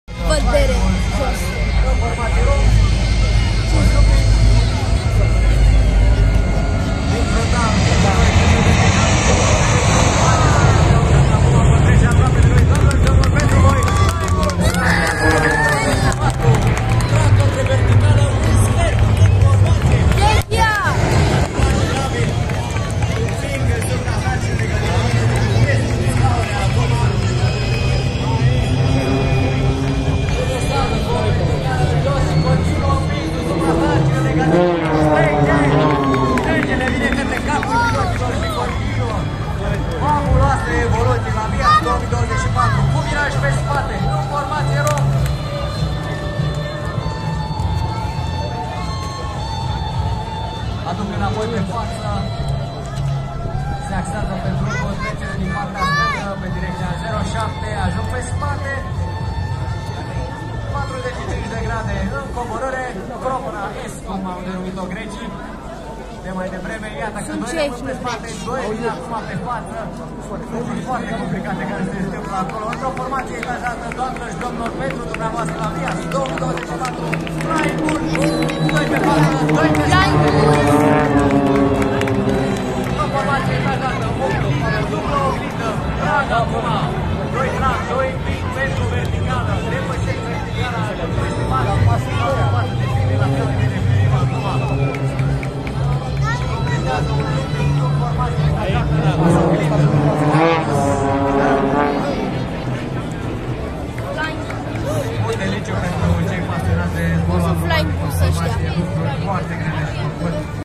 Flying Bulls XA-42 (Cehia 🇨🇿), BIAS 2024
Flying Bulls Aerobatics Team este o echipă civilă cu sediul în Republica Cehă, care zboară patru avioane de acrobație XtremeAir XA42, pictate în culorile băuturii energizante Red Bull. Toate avioanele echipei folosesc generatoare de fum alb.